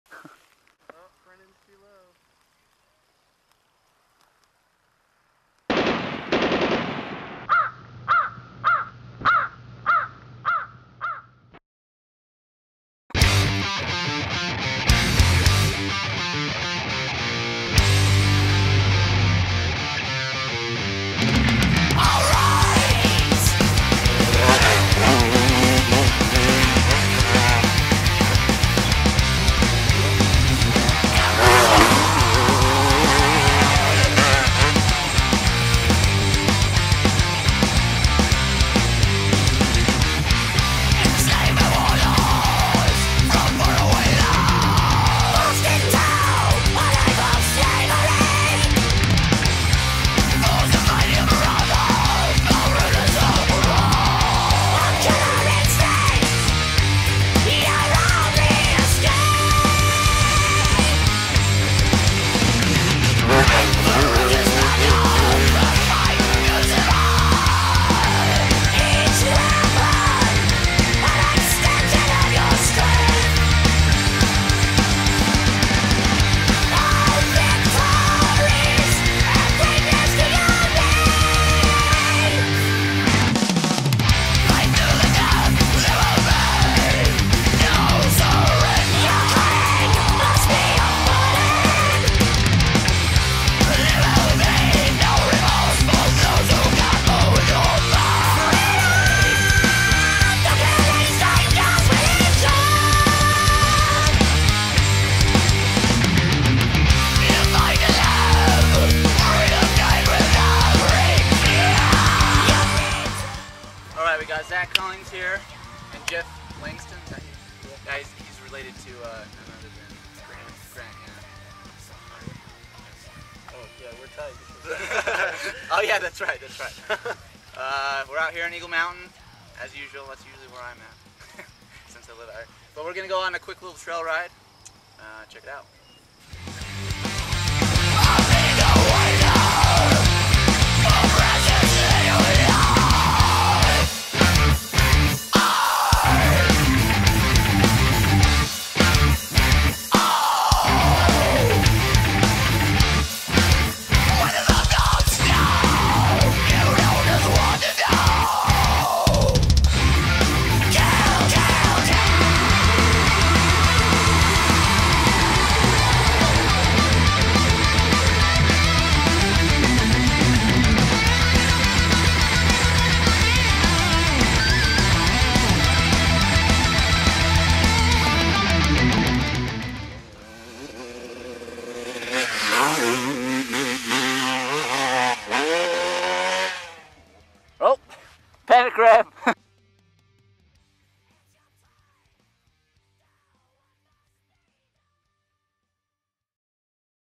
Eagle Mountain Freeride
Patrick and myself riding E.M. summer 2011. All the jumps in this vid from built mostly by Patrick.